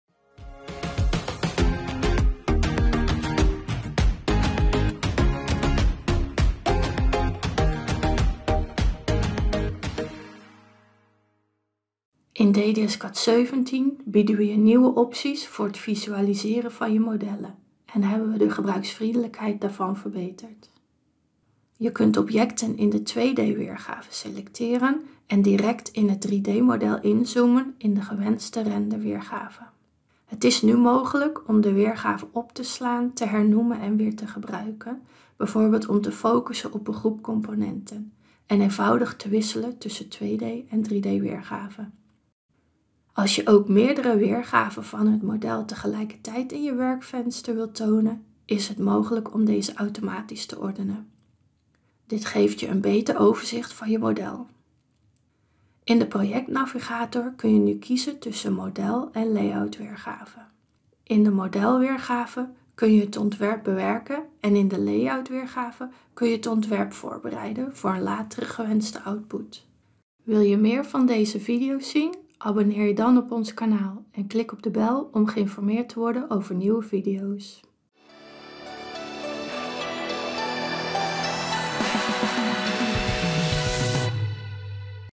DDScad 17 | Modellen gemakkelijker visualiseren
Modellen gemakkelijker visualiseren – in DDScad 17. Wil je DDScad live ervaren, een prijs aanvragen of meer informatie ontvangen?

In deze video:

In DDScad 17 bieden we je nieuwe opties voor het visualiseren van je modellen en hebben we ook de gebruiksvriendelijkheid daarvan verbeterd. Je kunt bijvoorbeeld objecten in de 2D-weergave selecteren en direct in het 3D-model inzoomen in de gewenste renderweergave, je kunt weergaven opslaan en je kunt nu ook doorsnedes en deelmodellen nog sneller en eenvoudiger aanmaken en beheren.